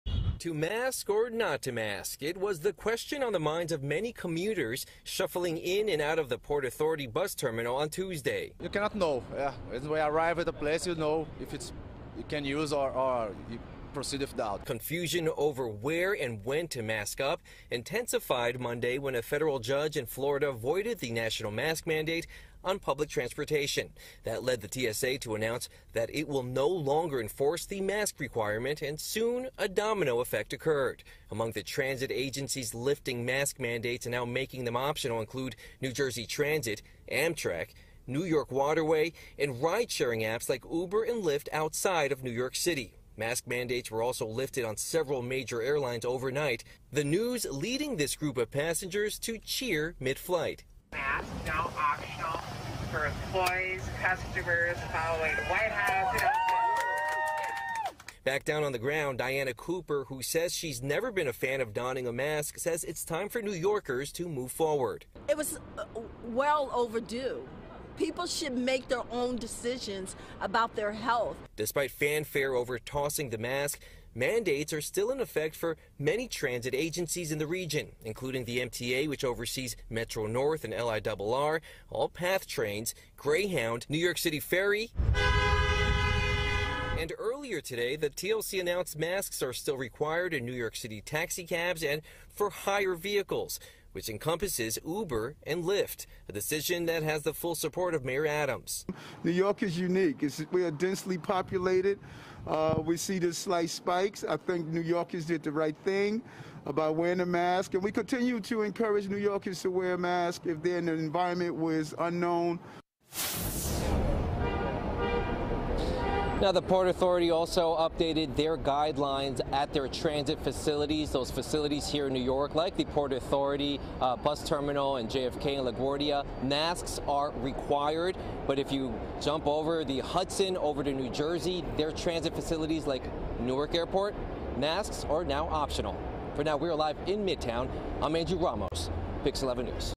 Mixed messages on mask requirements
A decision by a federal judge in Florida to throw out a national mask mandate in public transportation across the U.S. created a patchwork of rules Monday that vary by city and mode of transit.